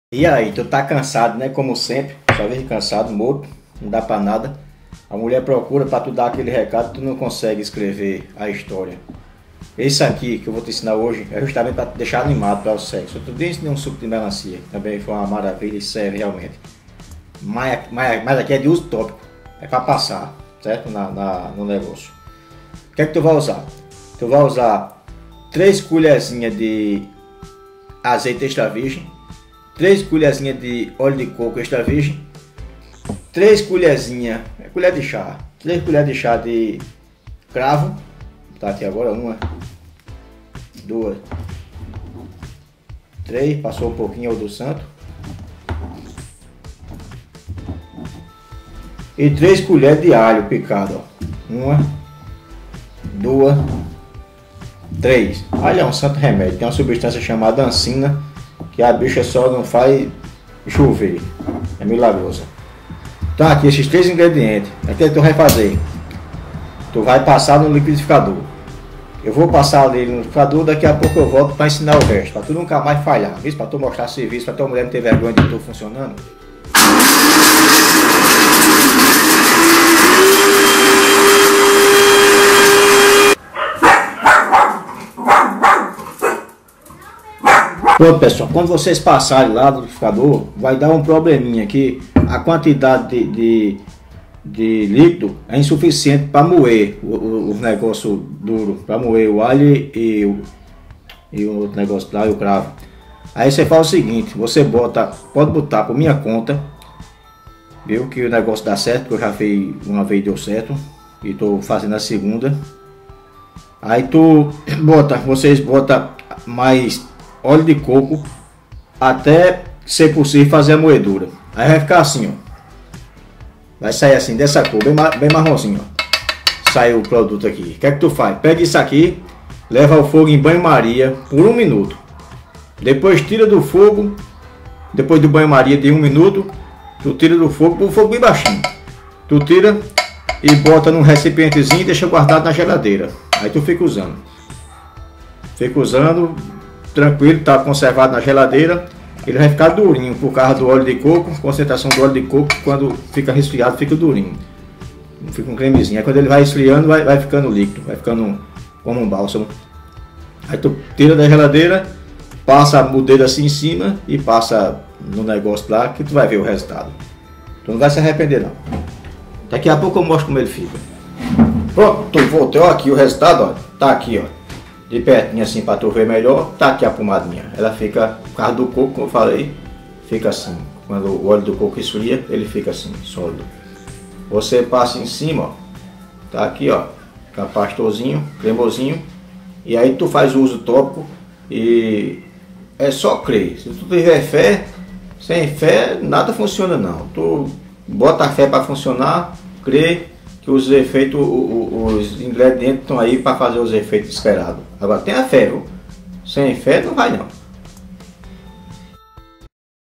O ALHO É UM VIAGRA NATURAL
Veja com o meio atrapalhado Dr. Alfrededão como aprontar uma pomada caseira com alho e cravo, dois alimentos inquestionavelmente bons para apimentar a relação.

Cultura, informação e entretenimento. Simbora Misturar.